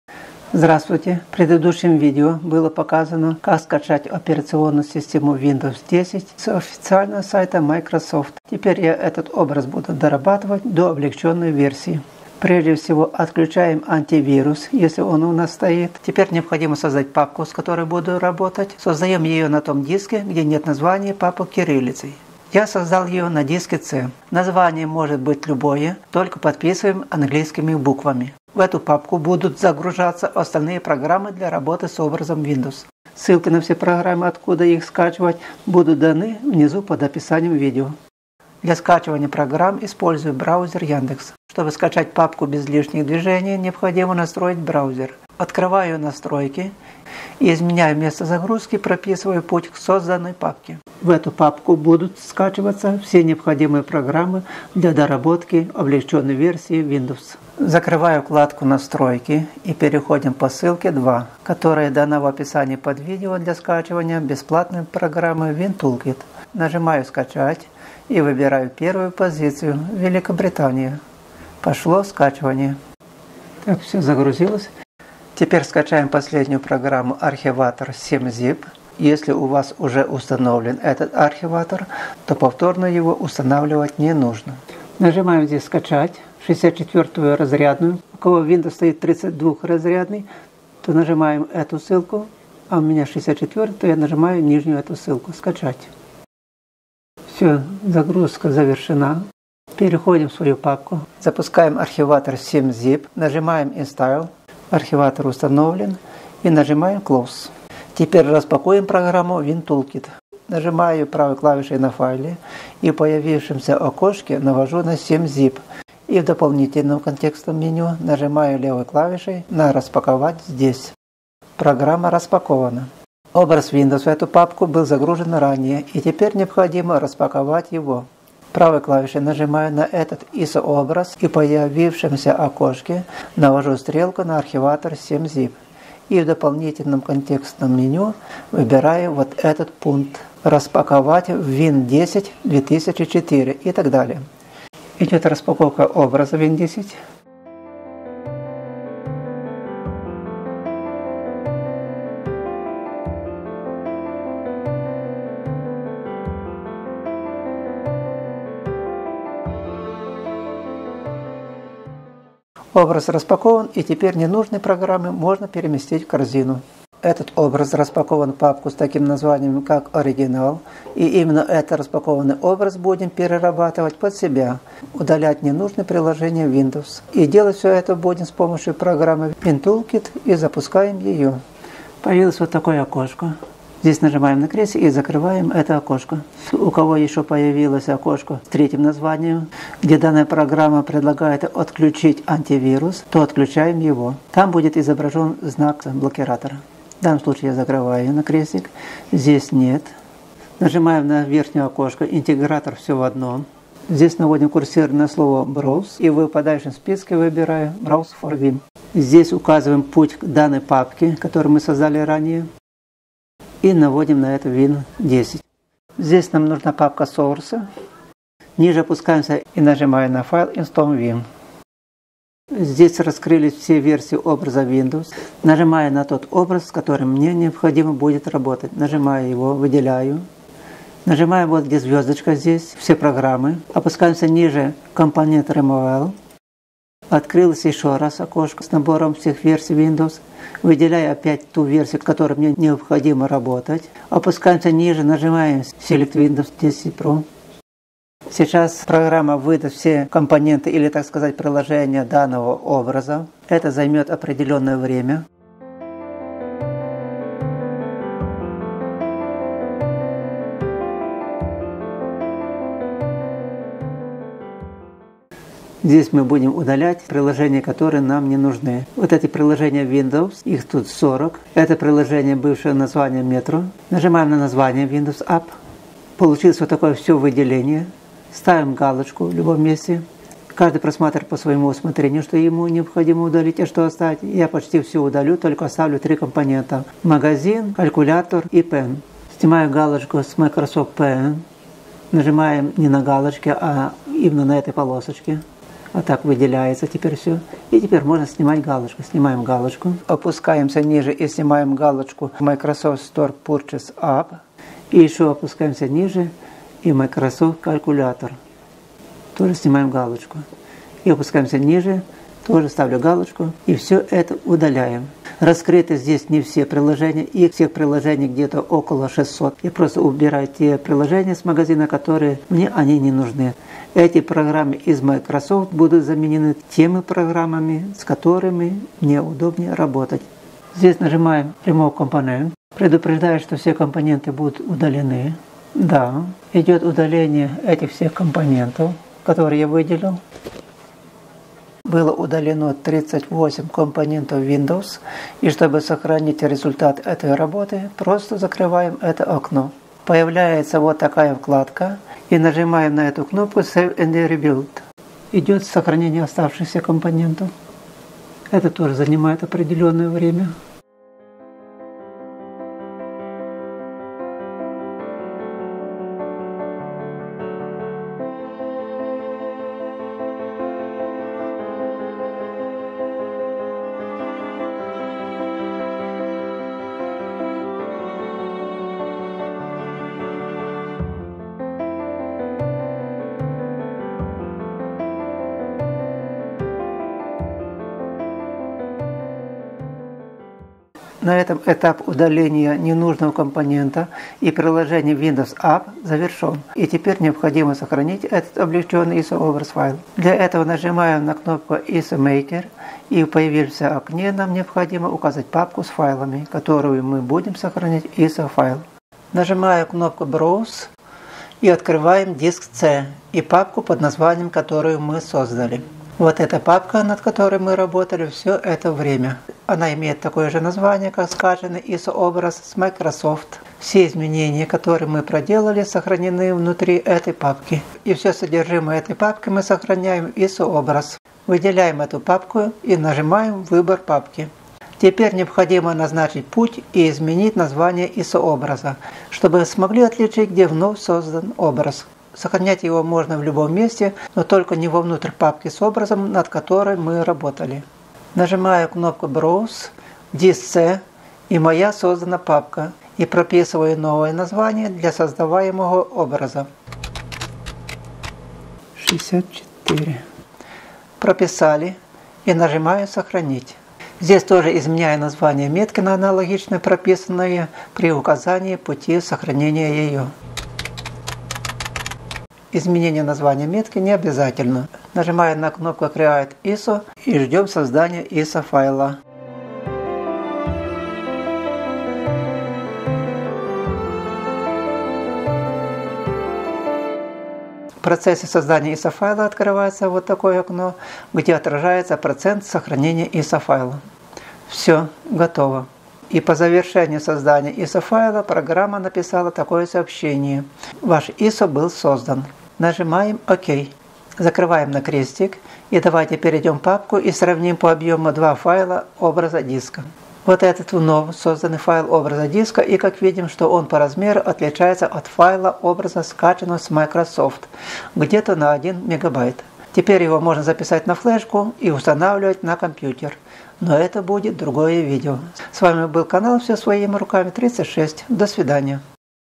Облегченная windows 10 lite своими руками,быстро работает на всех КП.
Облегченная версия windows 10 своими руками и для чего она нужна.В Windows Apps находится более 40 приложений с которыми мы не работаем.Поэтому,почти все приложения  удаляю,оставляю только необходимые.Смотрим видео и по желанию каждый сможет это попробовать проделать и увидить результат своей работы.Скачать windows 10 внизу имеется ссылка.Все программы используемые здесь бесплатные.